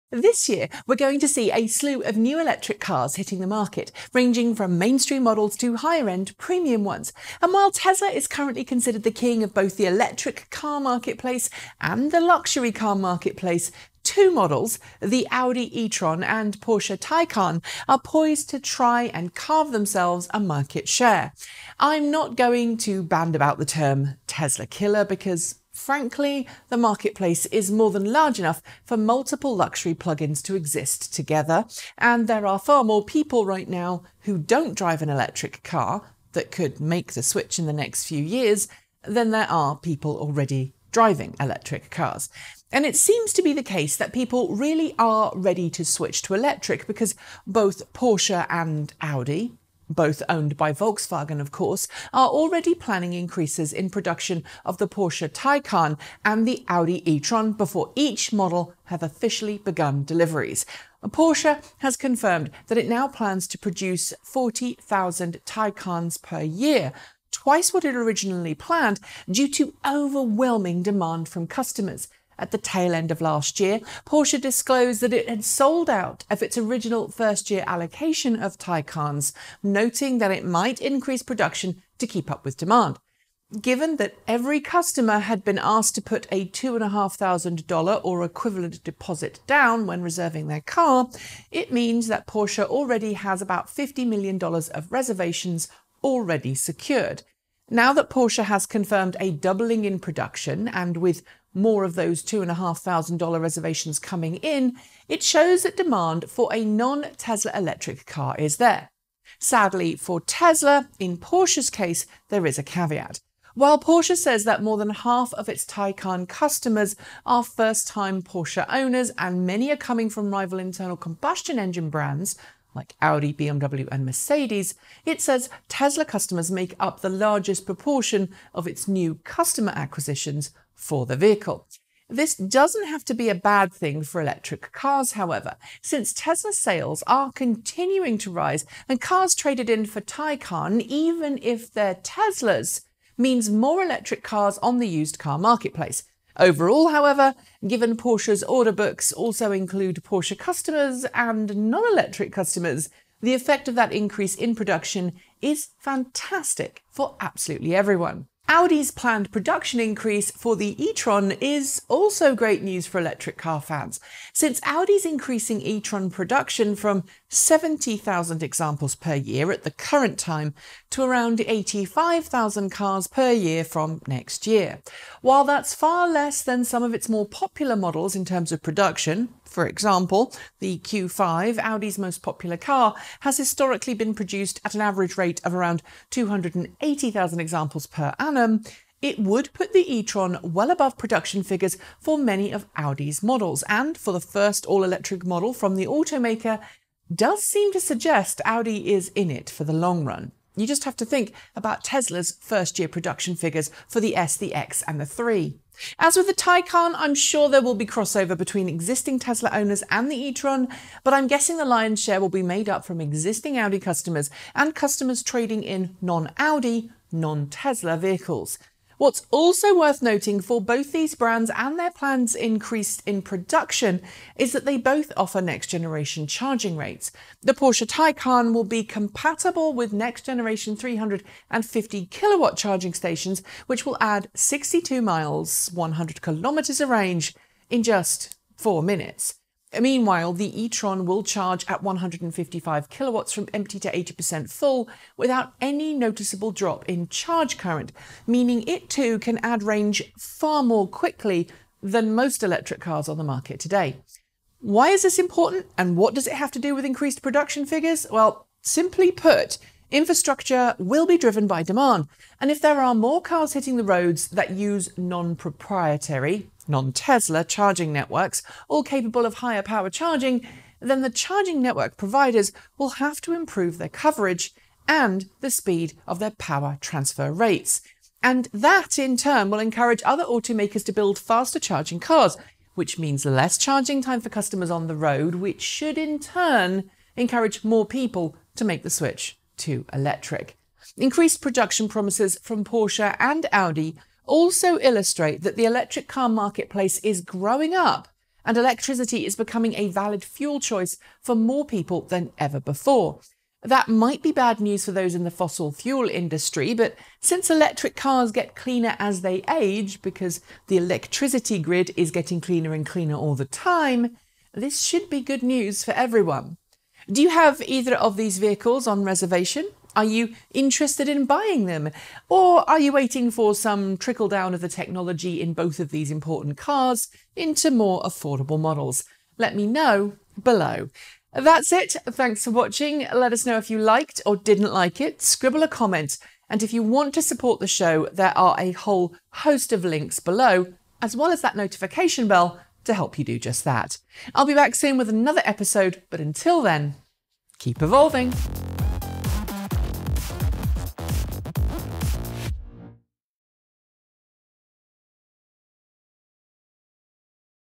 In Demand: Production Of Audi e-tron, Porsche Taycan Increased (And Why It’s Great News)
Audi and Porsche -- both Volkswagen companies -- are each bringing their first long-range all-electric models to market this year in the form of the Audi e-tron and Porsche Taycan.

Before they've even arrived at dealer lots however, both have announced they will be increasing production in the face of far higher demand than initially expected.

Here's why it's good news for Porsche, Audi, Tesla, and the EV world in general.